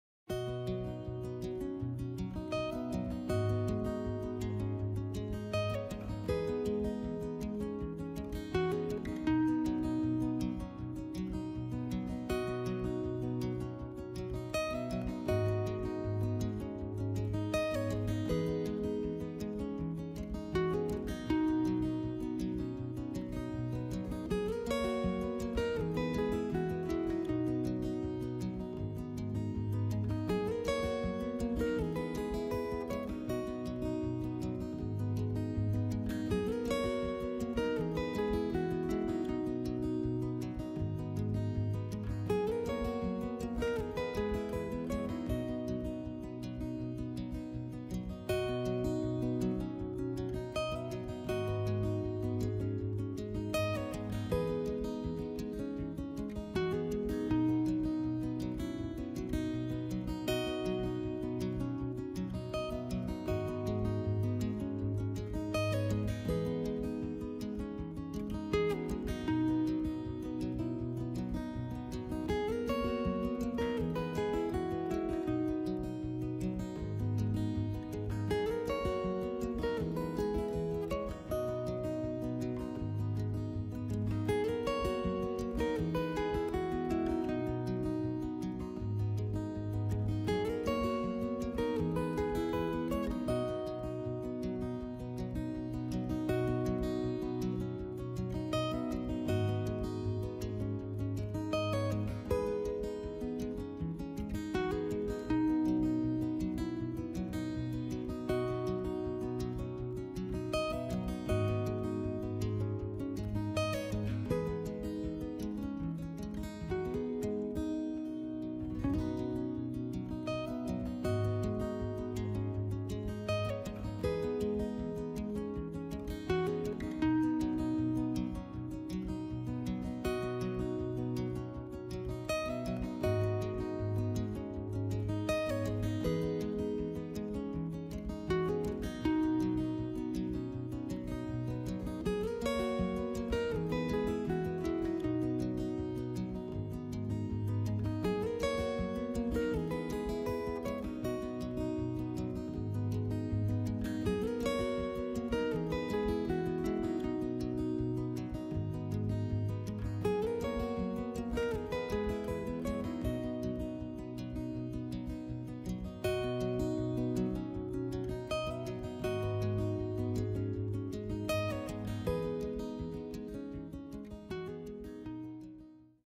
РИСУЕМ ПЕРСИДСКУЮ КОШКУ ПАСТЕЛЬЮ
Портрет пушистой кошки пастелью.